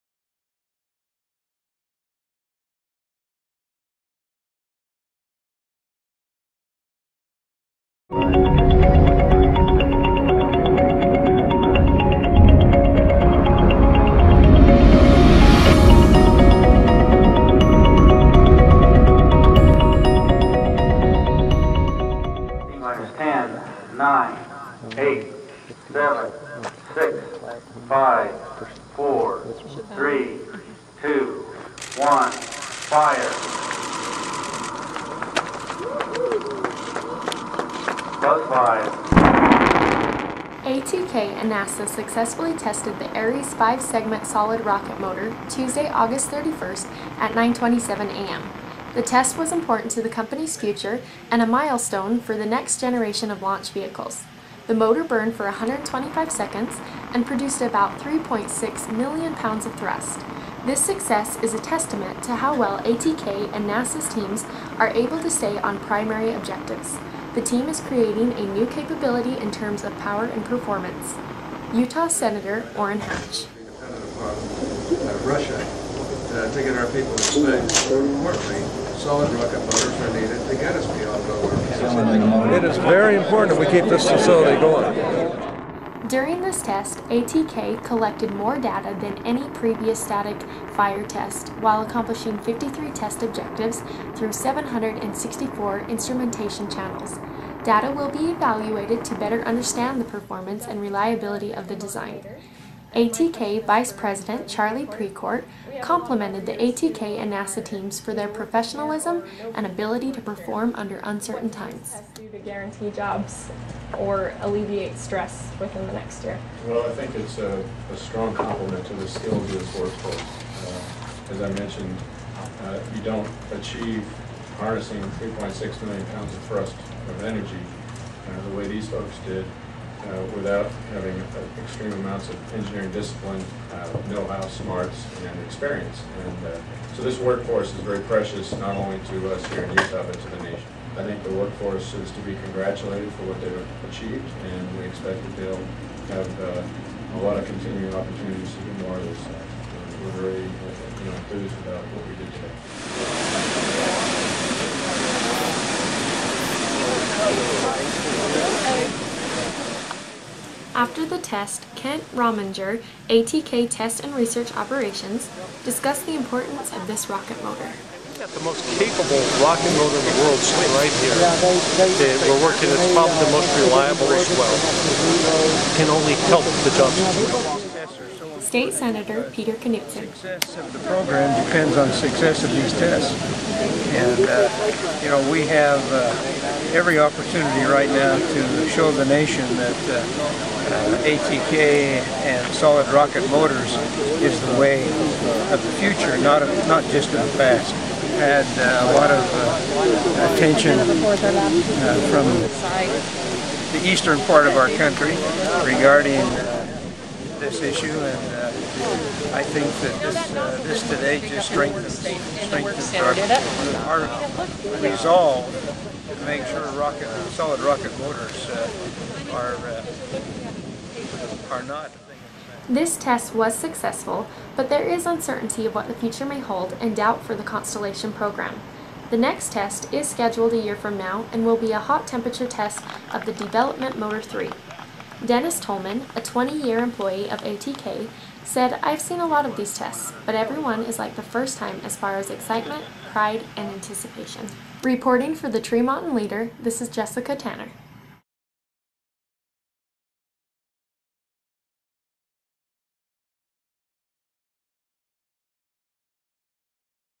ATK ARES DM-2 Static Test Firing 8-31-10
ATK successfully test fired their 5-segment ARES rocket motor Aug. 31, 2010 in Box Elder County. Here is a video report from the Tremonton Leader.